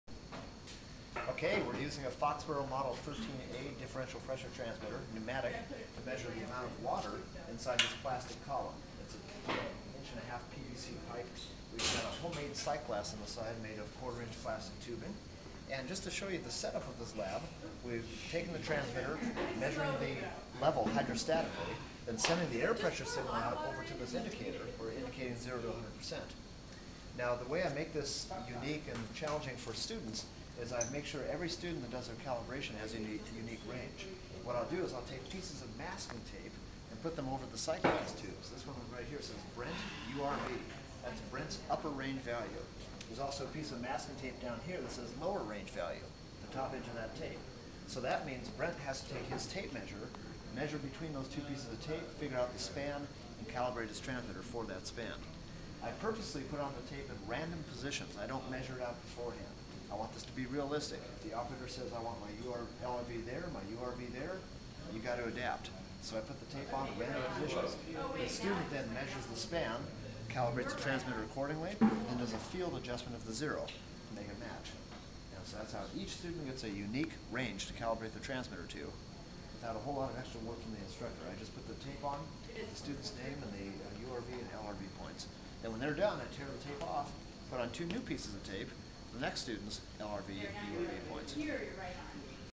Level measurement lab (pneumatic) -- random calibration ranges
A brief video discussing how students' water level measurement systems may be set up for random range values (pieces of masking tape placed on the sightglass).